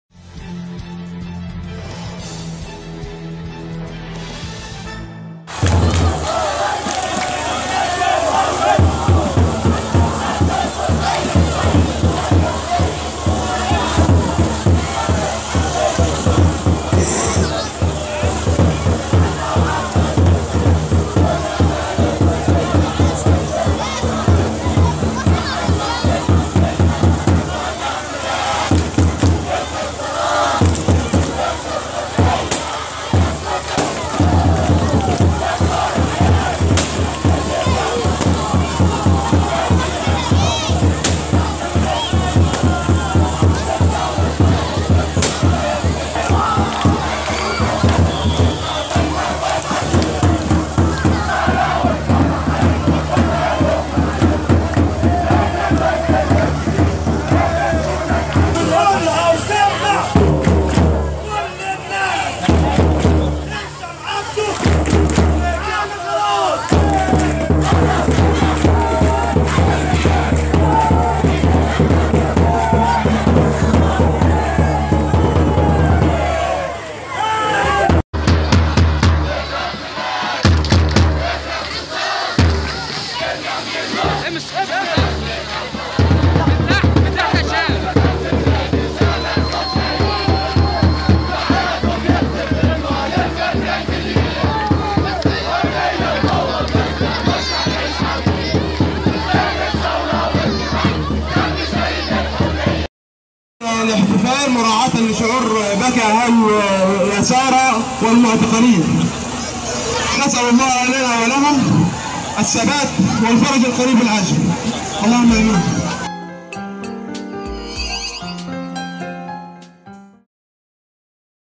رصد | قرية العدوة تحتفل بخروج معتقل لها منذ 7 اشهر
اهالي قرية العدوة مسقط راس الرئيس مرسي بالشرقية يحتفلون بخروج اول معتقل لهم من ابناء القرية بعد اعتقال دام 7 اشهر ...